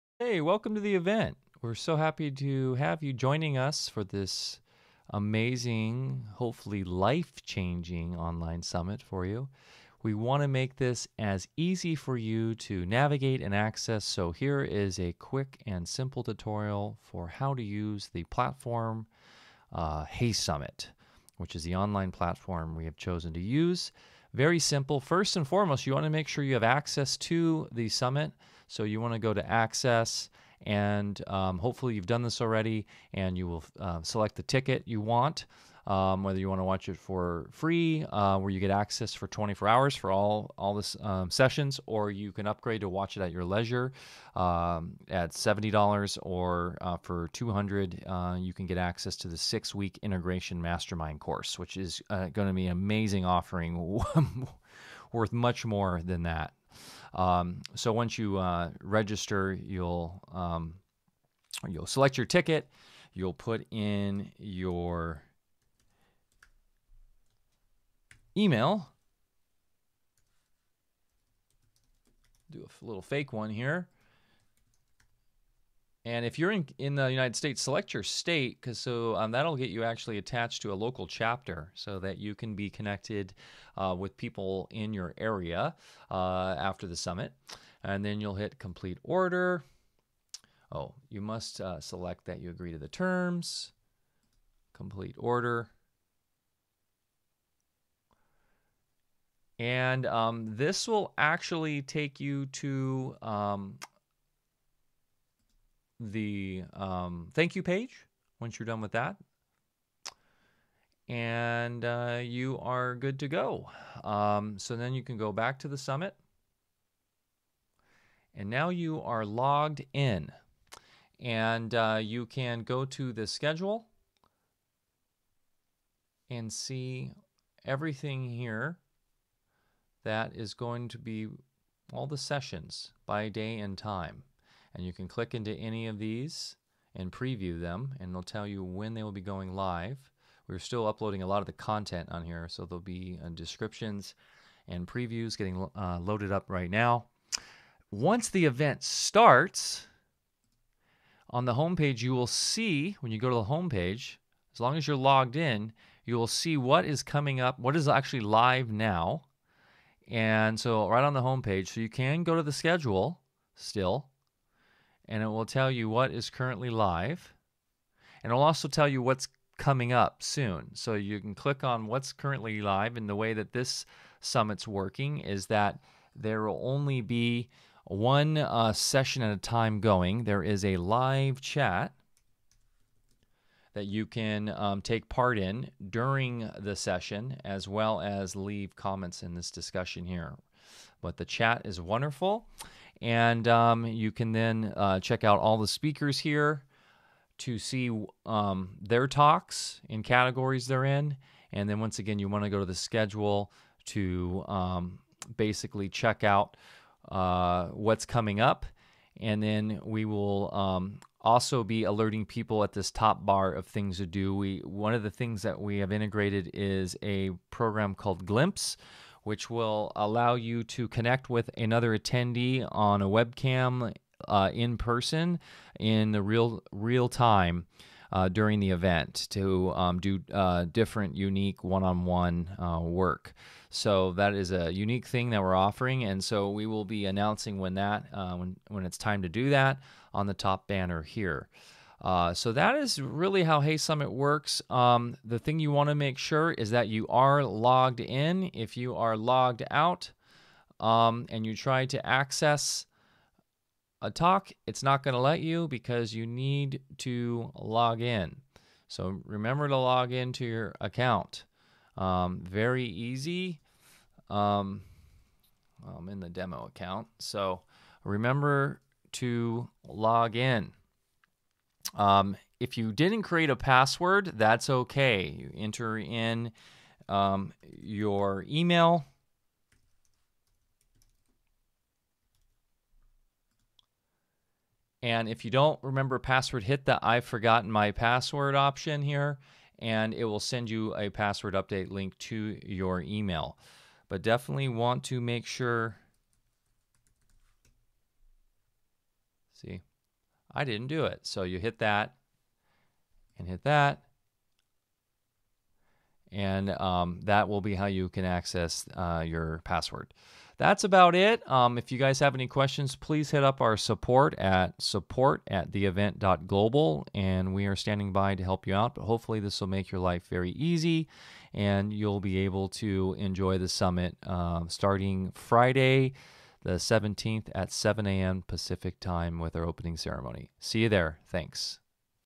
HeySummit Walkthrough
How to use our online summit platform.